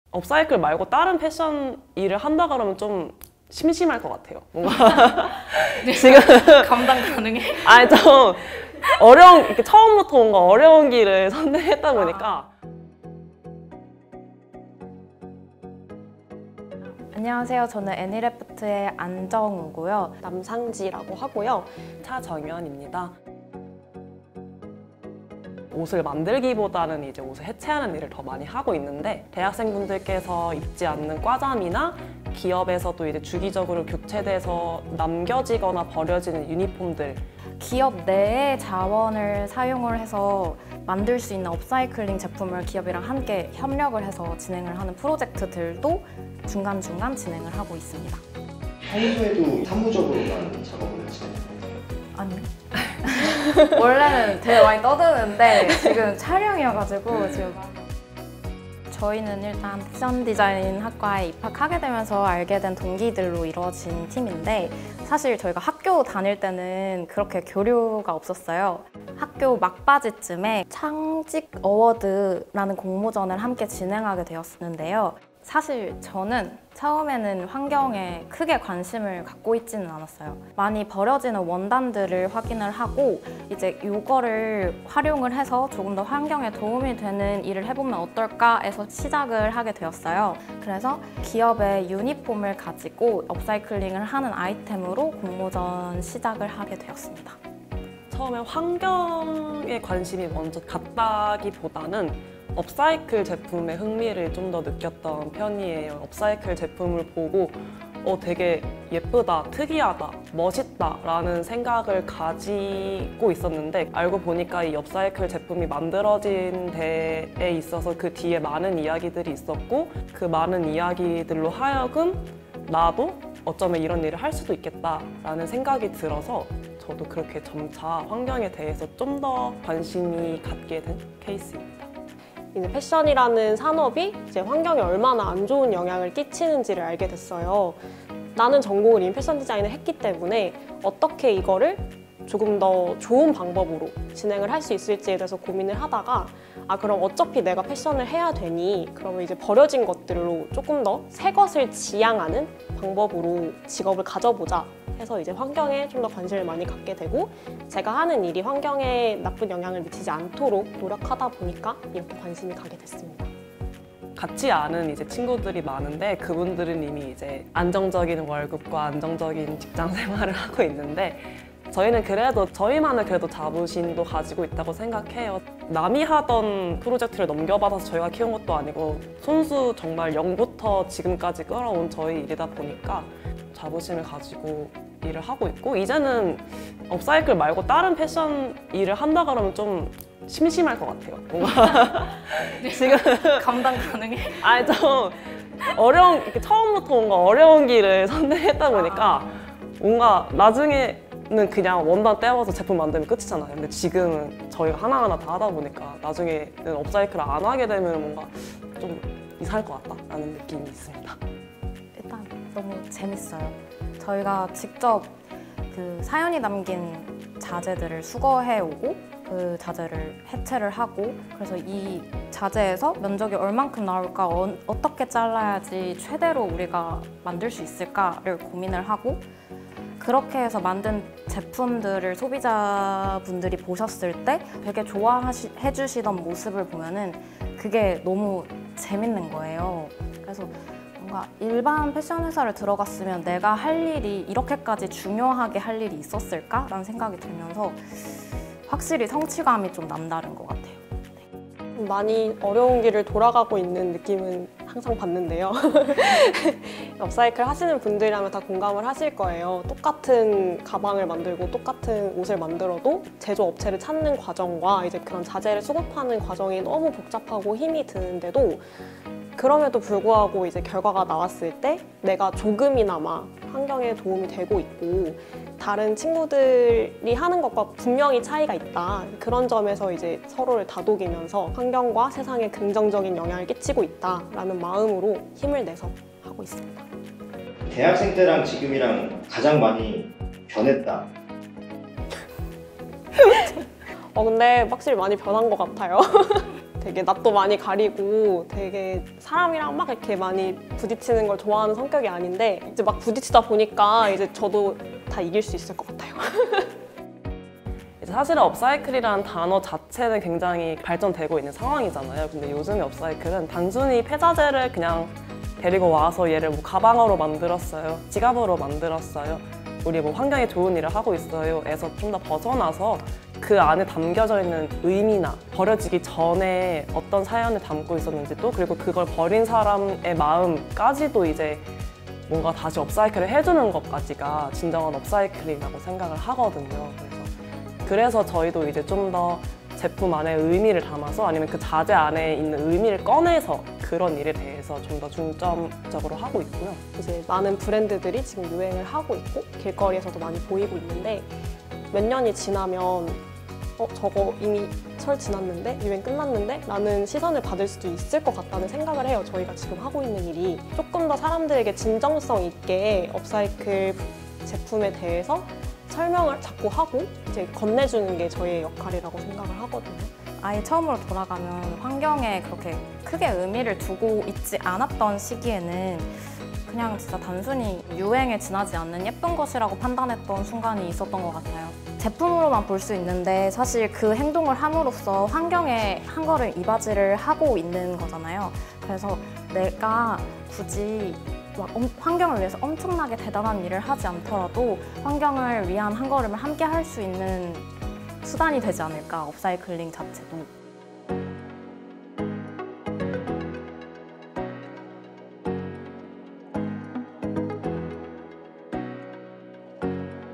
환경 창업으로 장롱 속 '과잠'을 추억의 아이템으로 만든 20대 업사이클링 브랜드
환경 창업으로 장롱속에 잊고 살았던 '과잠'을 새로운 의미의  추억의 아이템으로 만든 20대 대학교 동기 대표들이 있습니다!
업사이클링 브랜드 애니레프트의 3명의 공동대표님들을 만나보았는데요
 환경 창업으로 제품을 업사이클링 하는것이 아닌 버린 사람, 제품의 의미까지 업사이클링을 해야 진정한 업사이클링 이라는 이야기가 인상 깊은 인터뷰였습니다 :)
동기들과 함께 환경 창업 이라니...부럽네요😭

 꼭! 영상을 공유하실 땐 유튜브 링크를 사용해주세요 :)